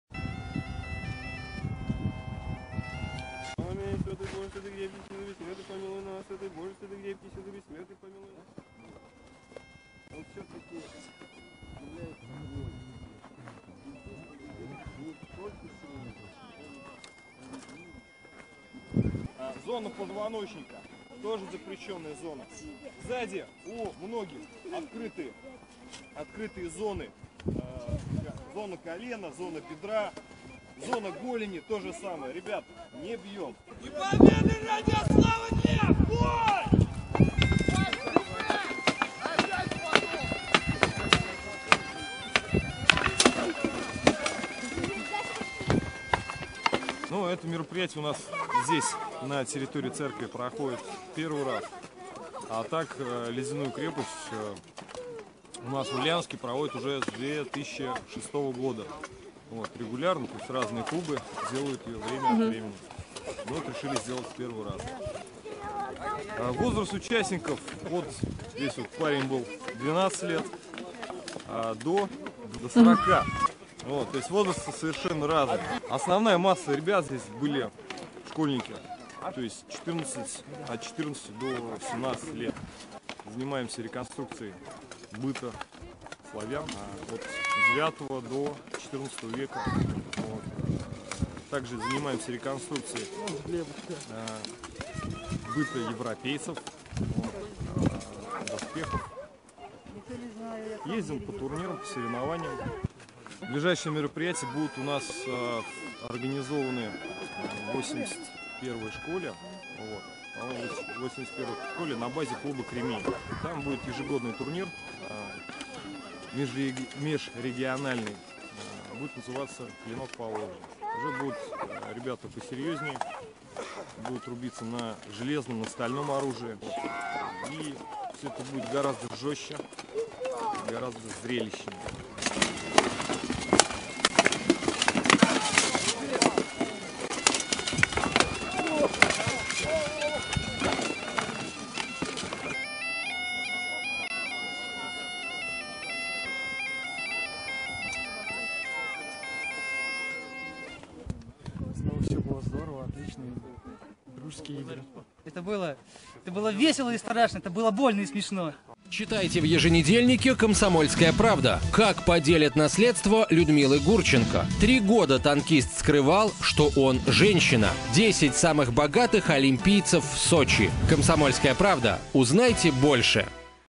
Исторические клубы Ульяновска сошлись в бою за ледяную крепость
На сайте еще и фото есть